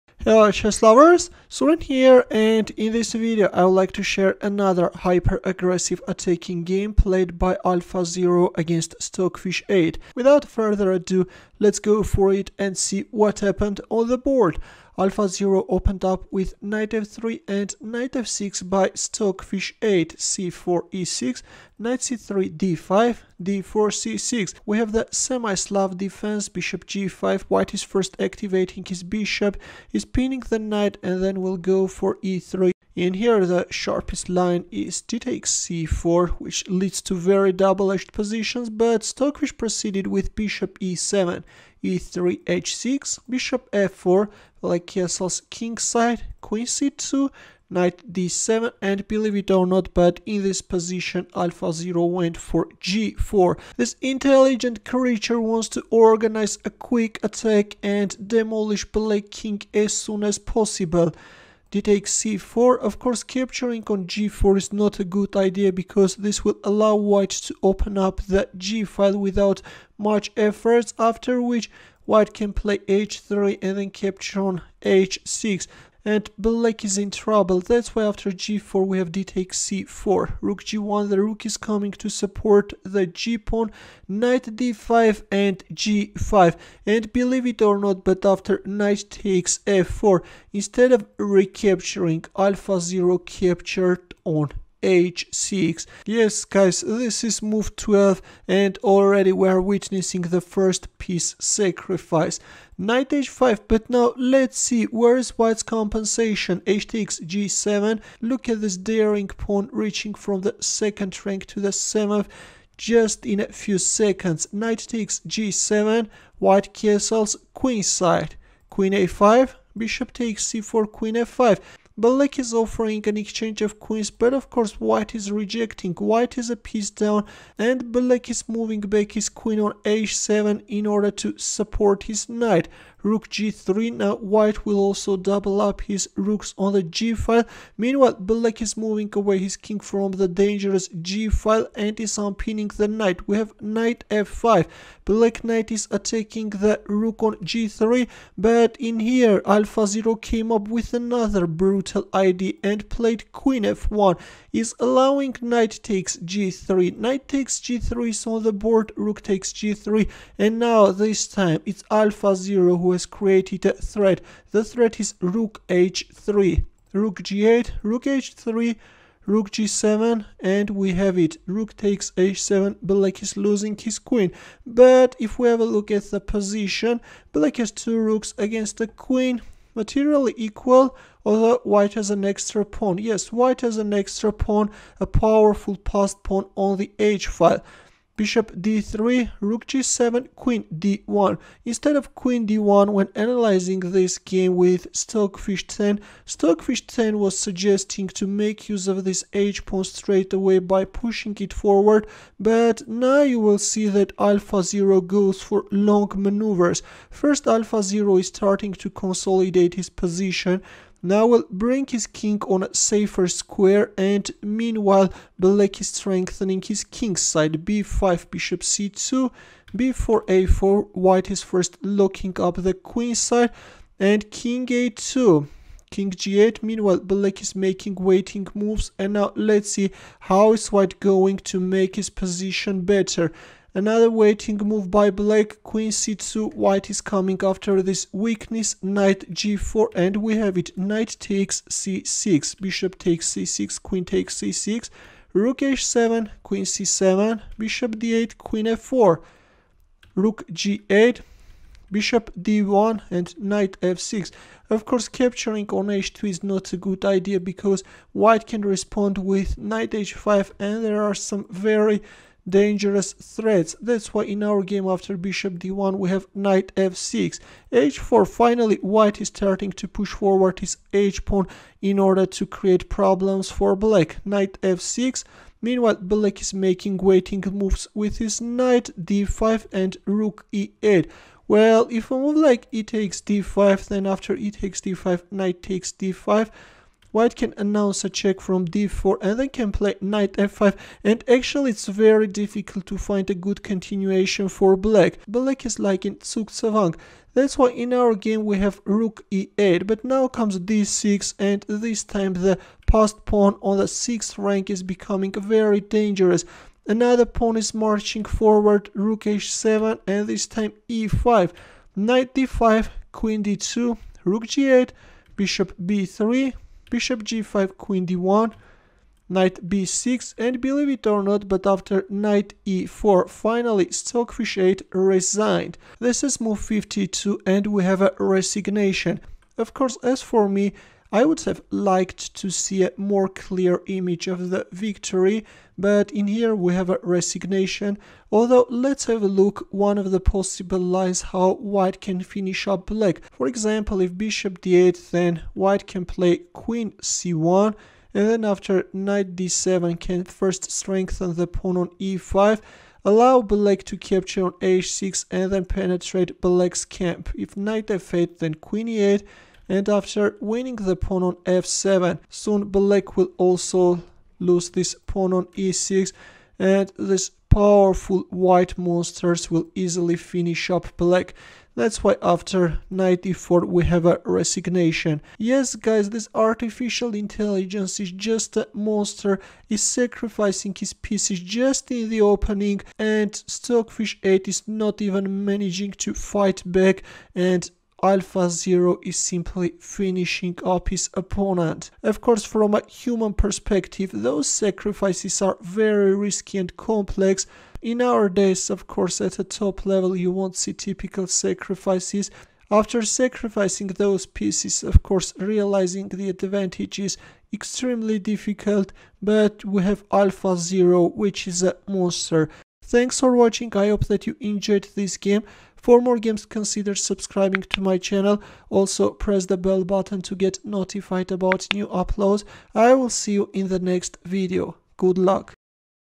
Unbelievable! AlphaZero Goes For A Piece Sacrifice On Move 12
In newly released games of DeepMind the updated AlphaZero crushes Stockfish 8 in a 1,000-game match, scoring +155 -6 =839.
Unlike the other games in this one AlphaZero goes for a quick piece sacrifice on move 12 in Semi-Slav defense and is getting great attacking chances! After some more daring moves AlphaZero is managing to gain an extra passed pawn on the h-file and after long manoeuvres is finally forcing Stockfish 8 to resign! A brilliant game worth of seeing!
AlphaZero vs. Stockfish 8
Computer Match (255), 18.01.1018
1. Nf3 Nf6 2. c4 e6 3. Nc3 d5 4. d4 c6 5. Bg5 Be7 6. e3 h6 7. Bf4 O-O 8. Qc2 Nbd7 9. g4 dxc4 10. Rg1 Nd5 11. g5 Nxf4 12. gxh6 Nh5 13. hxg7 Nxg7 14. O-O-O Qa5 15. Bxc4 Qf5 16. Qe2 Qh7 17. Rg3 Kh8 18. Rdg1 Nf5 19. Qf1 Nxg3 20. Rxg3 Rg8 21. Rh3 Rg7 22. Rxh7+ Rxh7 23. Bd3 Rg7 24. Qd1 Nf6 25. Ne5 Bd7 26. Qf3 Rf8 27.
Ne2 Kg8 28. a3 Be8 29. Kb1 a5 30. e4 b5 31. Bc2 b4 32. a4 Kh8 33. Ka2 Kg8 34.Ng3 Rg5 35. Qd1 Kh8 36. Bb3 Rg7 37. Qc2 Ng4 38. Nxc6 Bxc6 39. Qxc6 Rh7 40. Qc7 Bd8 41. Qf4 Rg8 42. Bd1 Nf6 43. h4 Nd7 44. h5 Nf6 45. d5 Re8 46. d6 Rg7 47. h6 Rh7 48. e5 Nd5 49. Qd2 Rg8 50. Bb3 Bg5 51. Qd1 Nb6 52. Ne4
 1-0